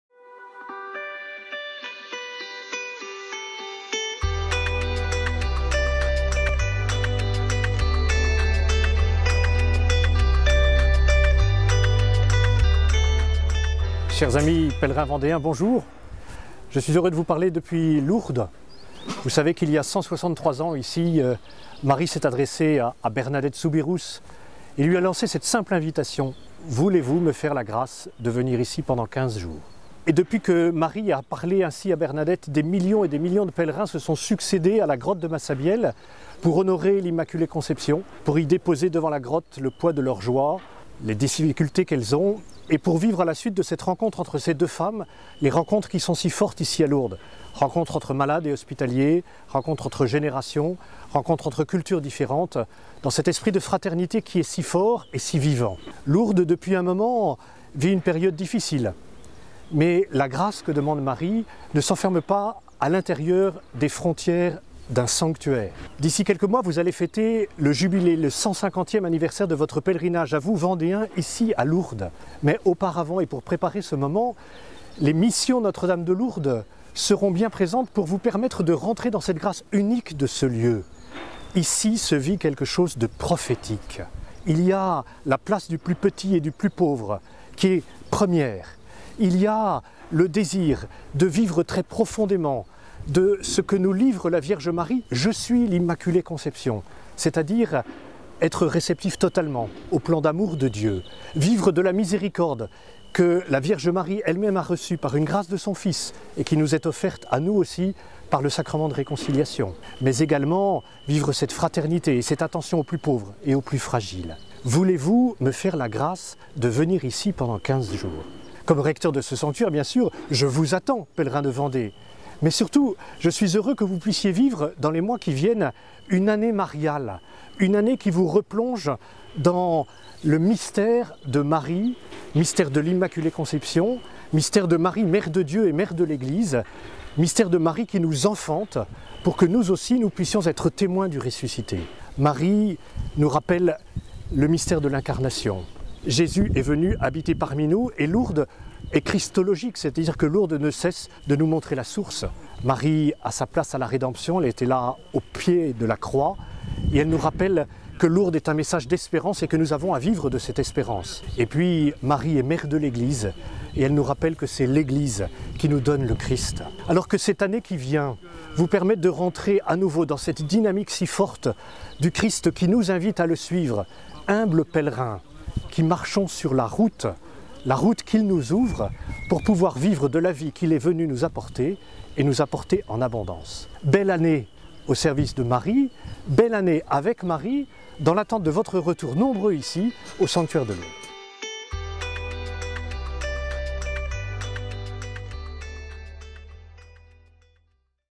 Lourdes chez vous en 2022 !
Le père Olivier Ribadeau Dumas, recteur du sanctuaire de Notre-Dame de Lourdes, nous invite, pour cette année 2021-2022, à vivre pleinement l'accueil des Missions de Lourdes en Vendée. Un évènement qui se déroulera dans le cadre de l'Année Mariale qui débute le 17 octobre prochain dans le diocèse de Luçon.

Image : Sanctuaire Notre-Dame de Lourdes